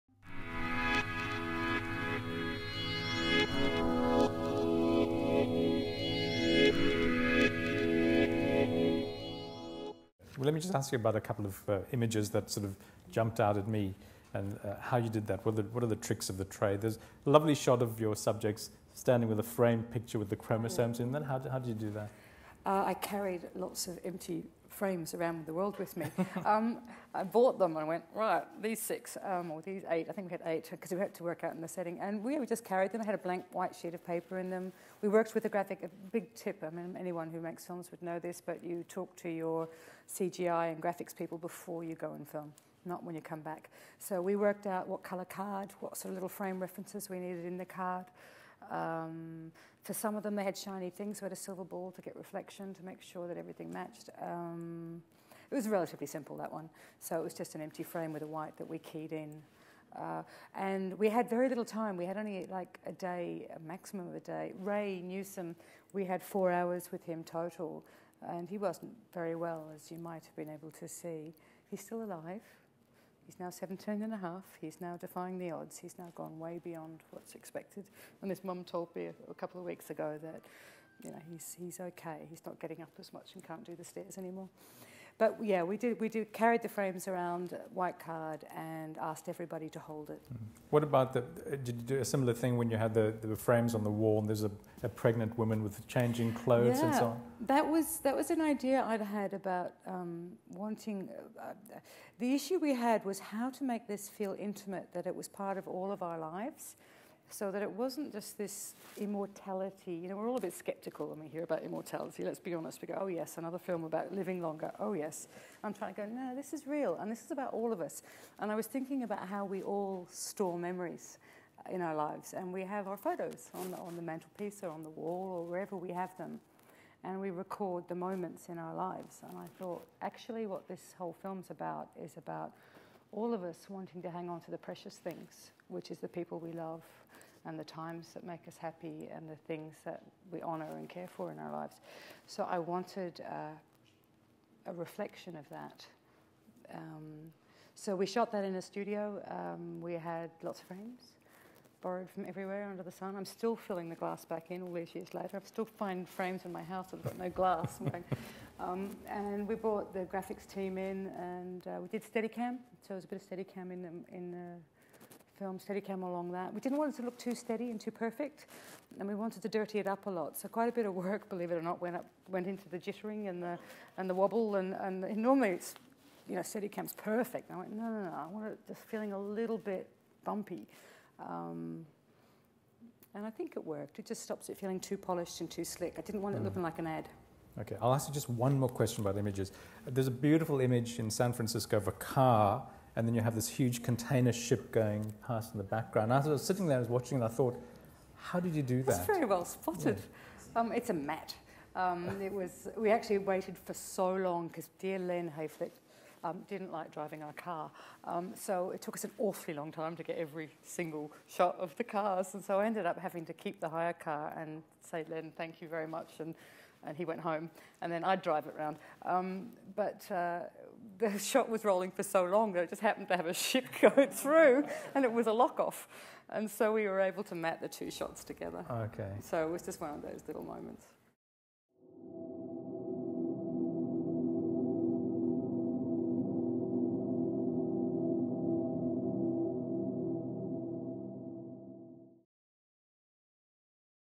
Immortal - Sonya Pemberton on the visual aspects of the film
Producer Sonya Pemberton talks about the making of the documentary 'Immortal', screened at RiAus, Adelaide, as part of the RiAus collaboration with SBS on its Science Season series 'Secrets of the Human Body'.